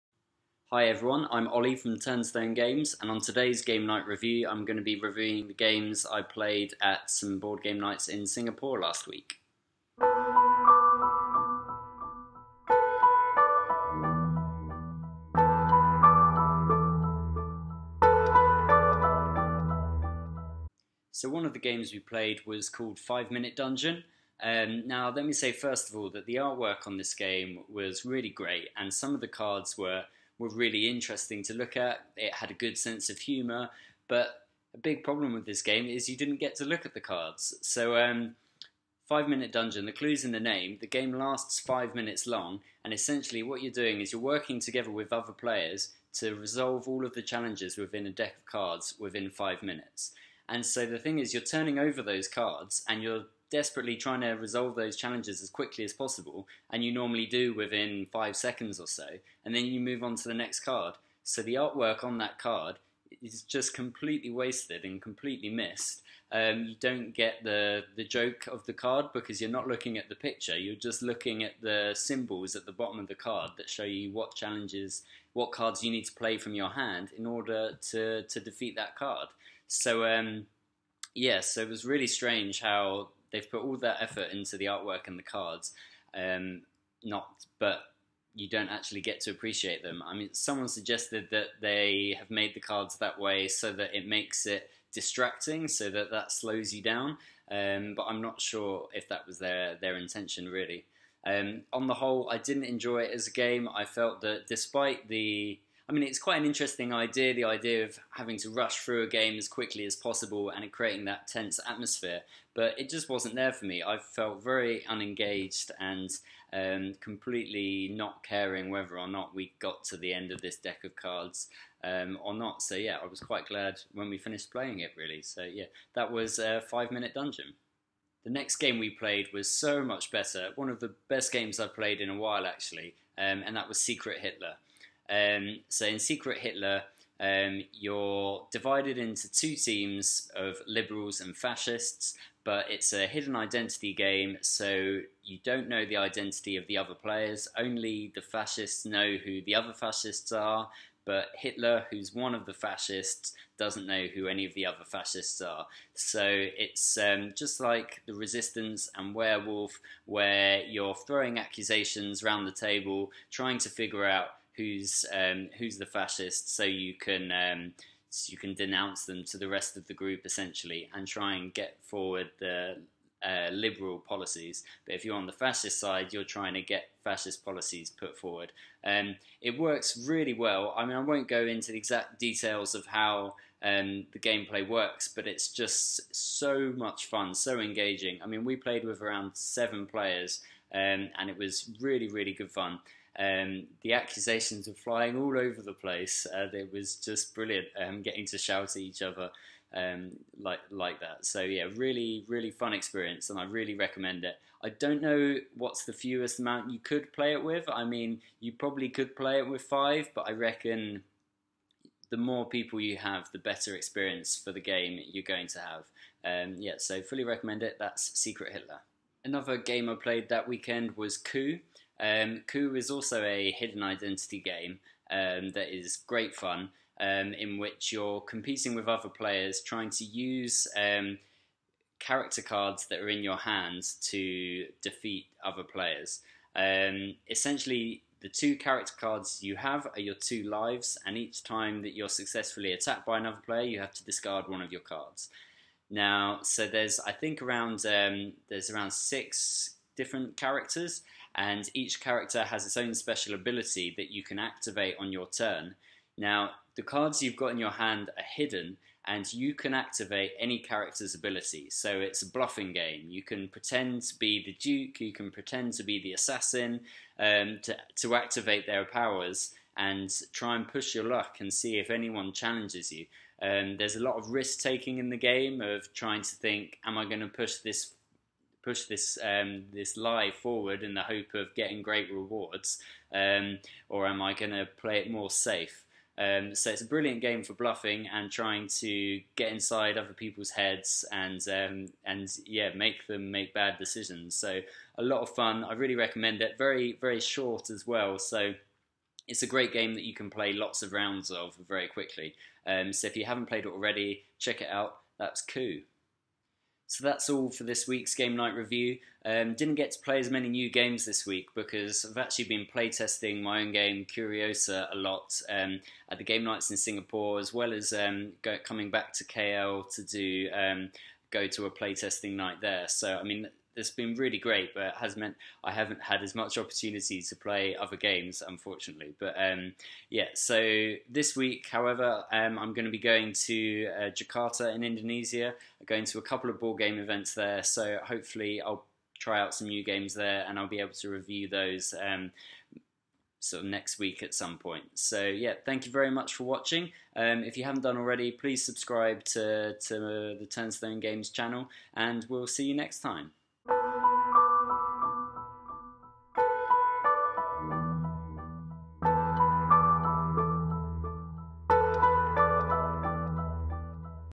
Board Game Night Review #3 (5 Minute Dungeon, Secret Hitler, Coup)
A review of the games I played last week at board game nights in Singapore and KL. Includes 5 Minute Dungeon, Secret Hitler and Coup.